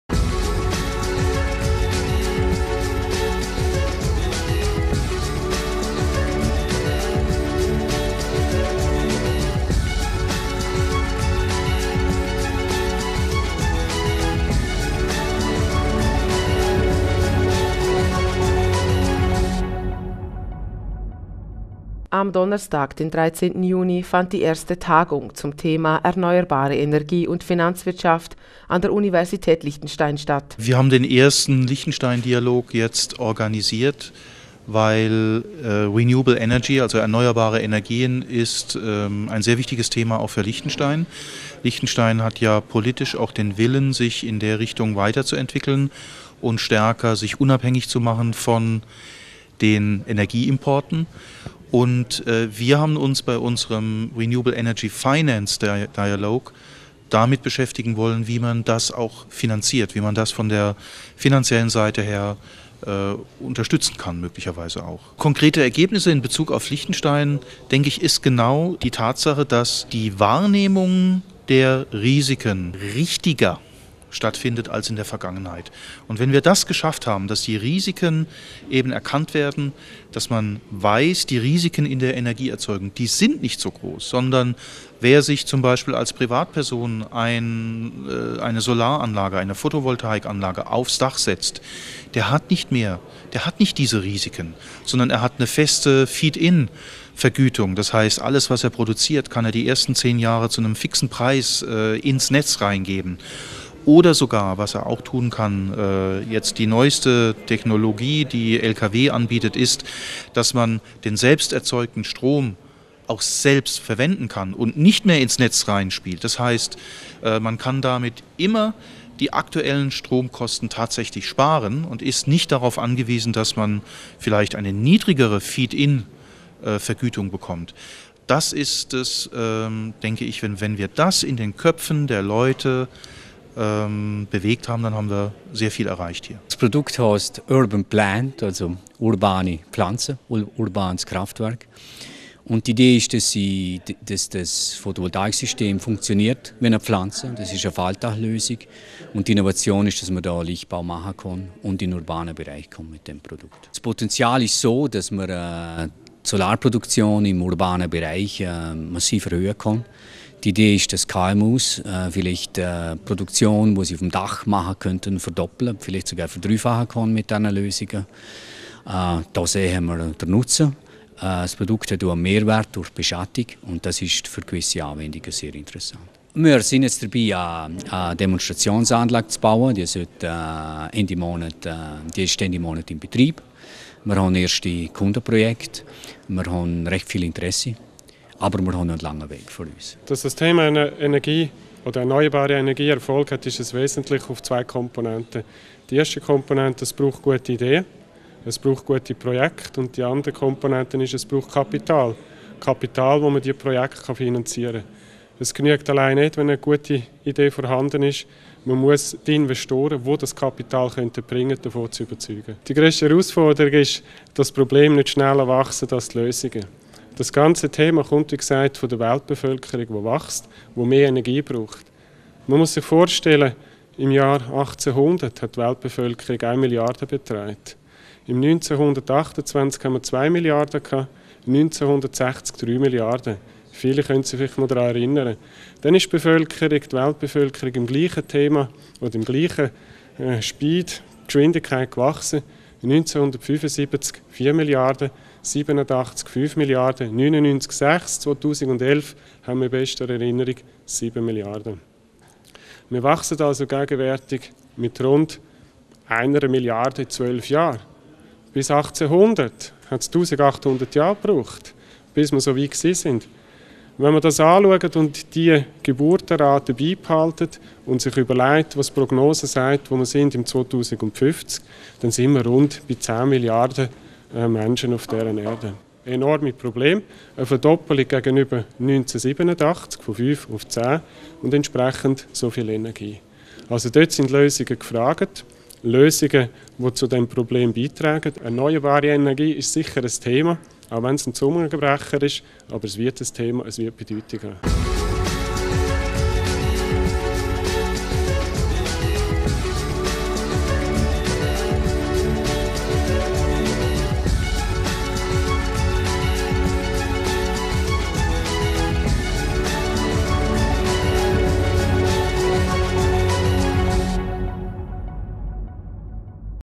1st Renewable Energy Finance Dialogue - Universität Liechtenstein
The first ever conference on the topic of "Renewable energy and finance" at the University of Liechtensteintook place on Thursday, 13 June 2013.

The event focused on the links between changes in society, the technological possibilities of renewable energies, and the challenges arising from these for the financial industry.

This new conference event represents a first step towards creating a platform for drawing together Liechtenstein's financial industry and the increasingly important renewable energy sector.